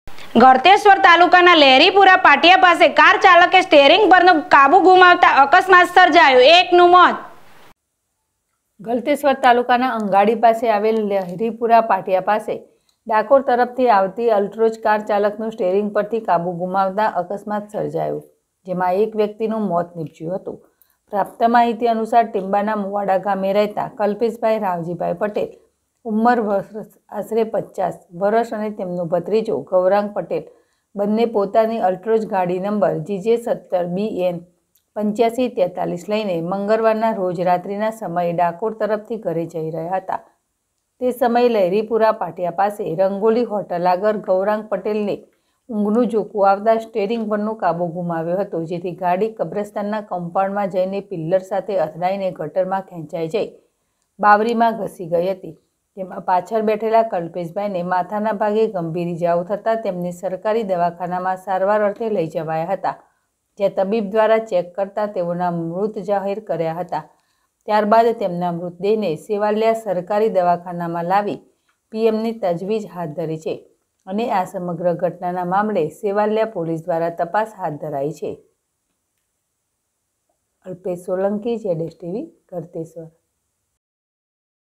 ગળતેશ્વર તાલુકાના લહેરીપુરા પાટિયા પાસે કાર ચાલકે સ્ટીયરિંગ પરનો કાબુ ગુમાવતા અકસ્માત સર્જાયો
ગળતેશ્વર તાલુકાના લહેરીપુરા પાટિયા પાસે કાર ચાલકે સ્ટીયરિંગ પરનો કાબુ ગુમાવતા અકસ્માત સર્જાયો, એકનું મોત.